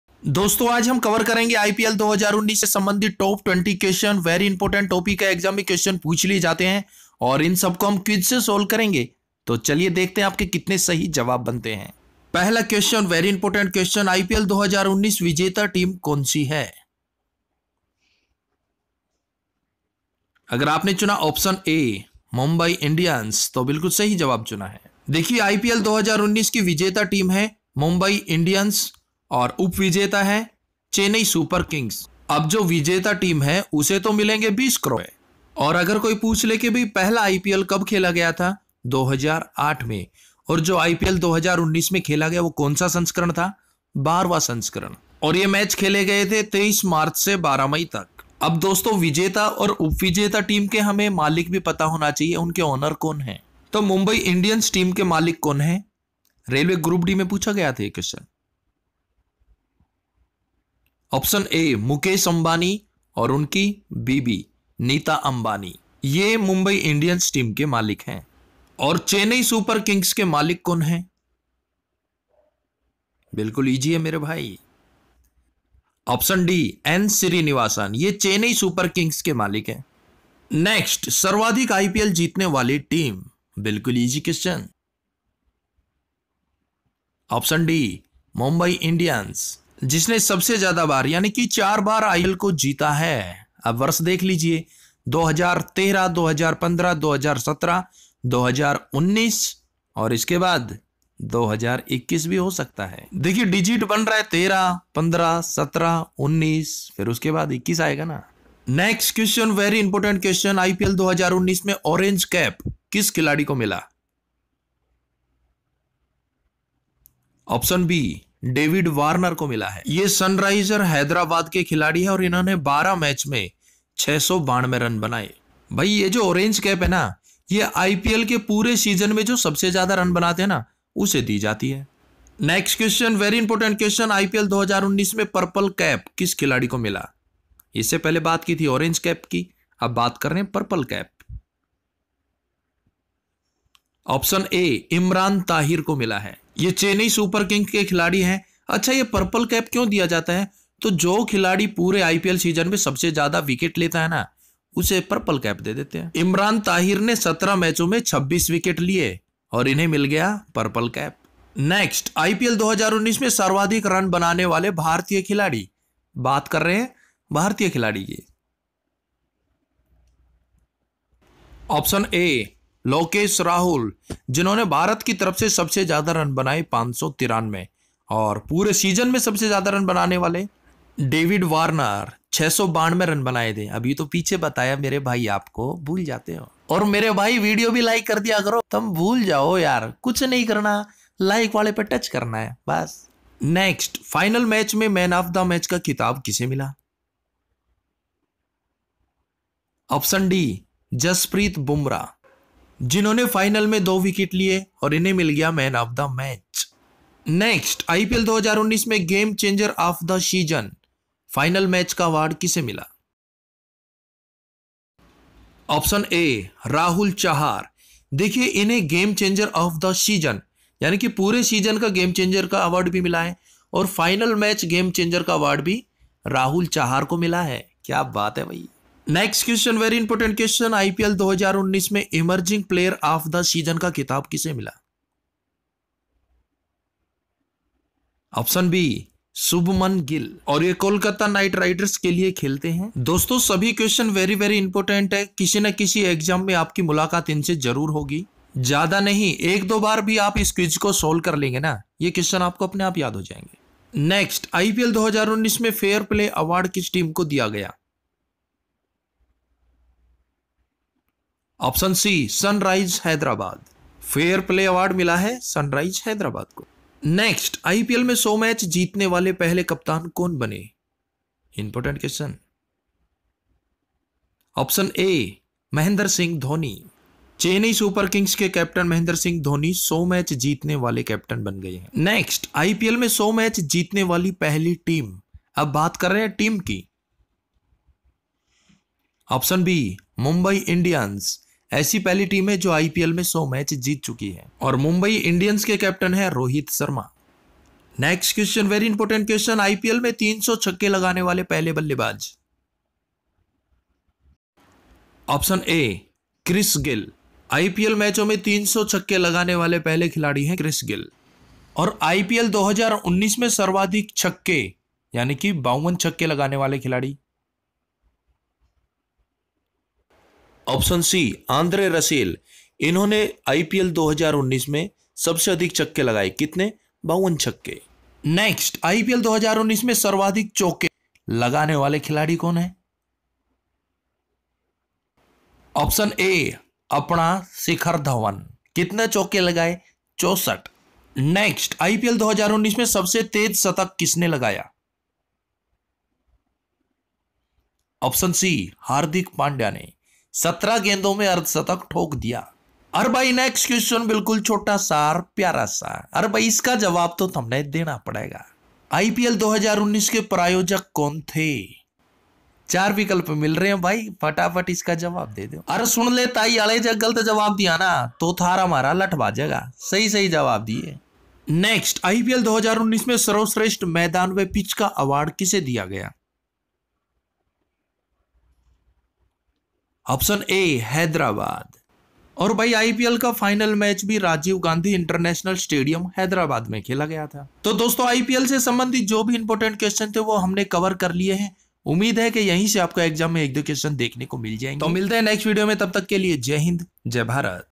IPL 2019 TOP important Question | आईपीएल 2019 | Current affairs 2019 | Gk mahatmaji
1. IPL 2019 TOP important Question answer, Current affairs 2019 for all exam